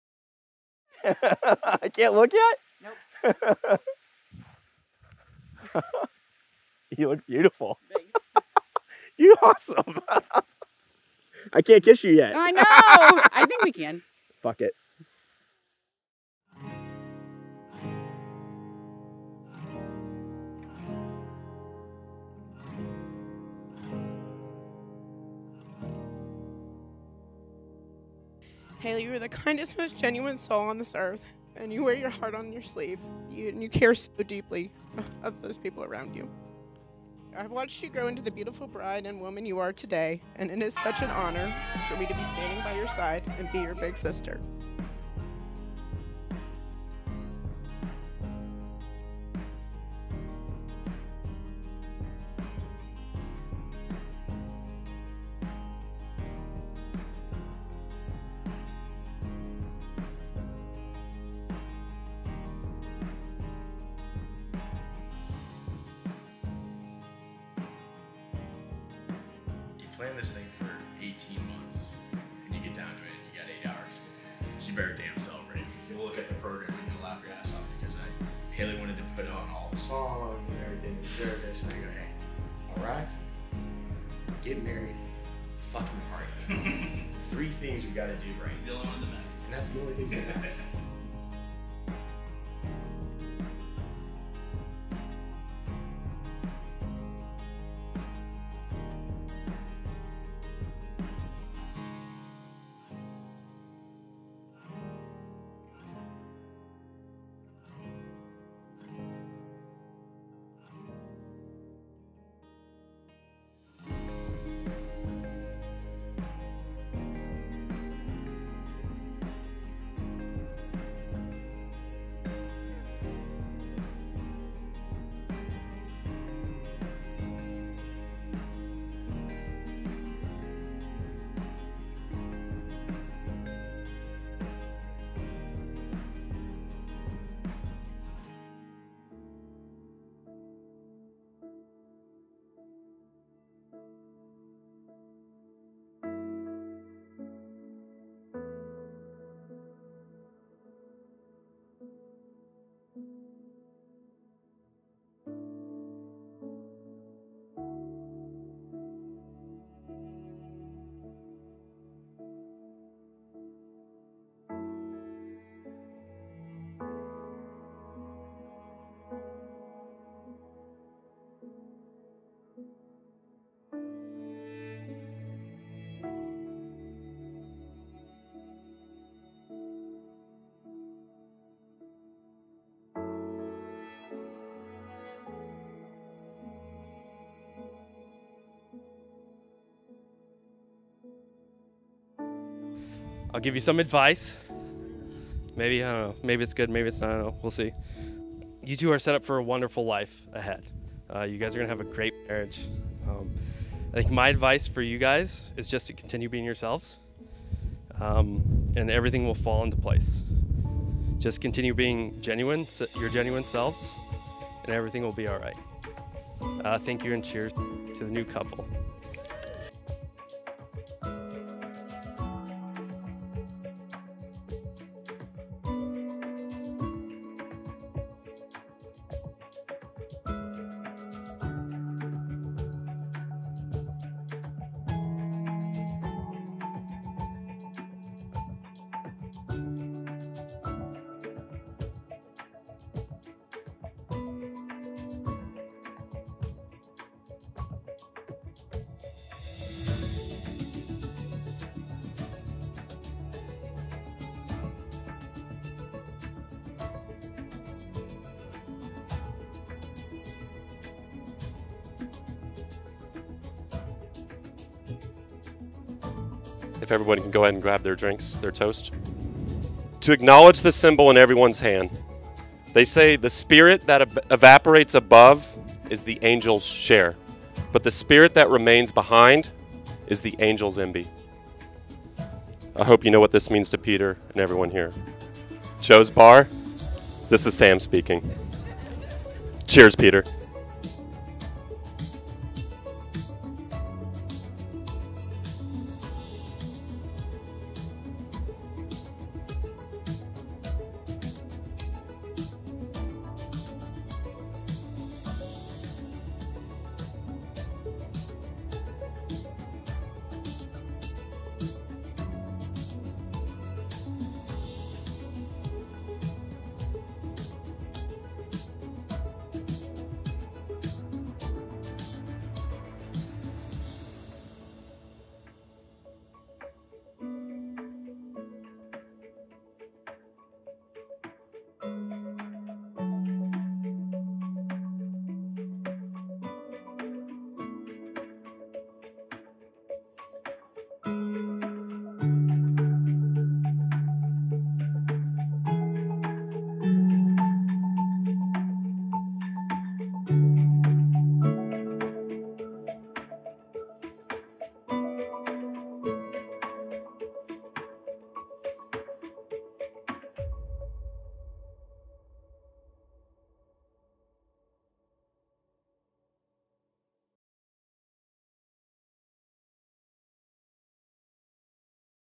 Craig & Hayley // Villa Antonia
Craig and Hayley // Villa Antonia, Austin, Texas

Capturing the love in this one. They were so authentic to themselves. Hayley and Craig laughed, they cried, they smiled and they had a great time.

- Gear -

A7IV
Sony G-Master 24mm-70mm
Ronin RS2
Rode Go Wireless Lav Mics